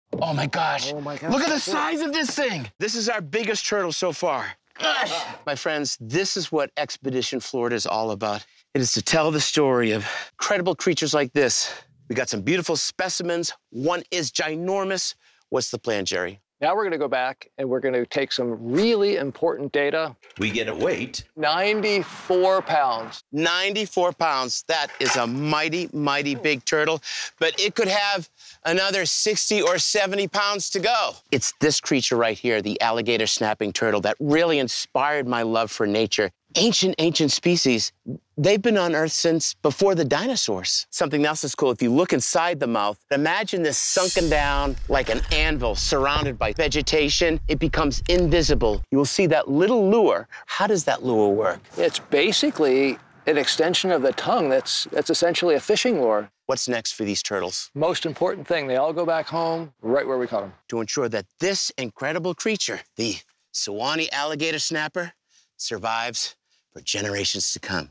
A Living Fossil with a Built-In Fishing Lure!
@jeffcorwingoeswild captures an alligator snapping turtle deep in the Florida Everglades! These prehistoric reptiles have roamed the Earth since before the dinosaurs! Watch as Jeff weighs it, checks its vitals, and reveals the wild secret behind how it lures prey, with a worm-like tongue!